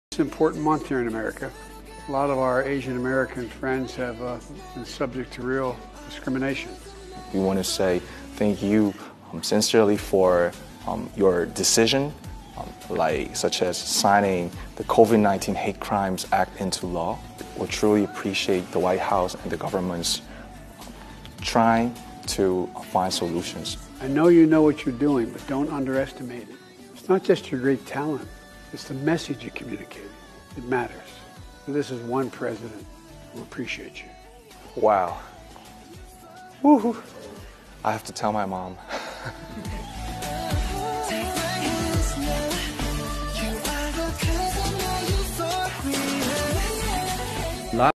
president biden telling bangtan how much he appreciates them
(From : YouTube - The WhiteHouse President Biden and Vice President Harris Welcome BTS to the White House)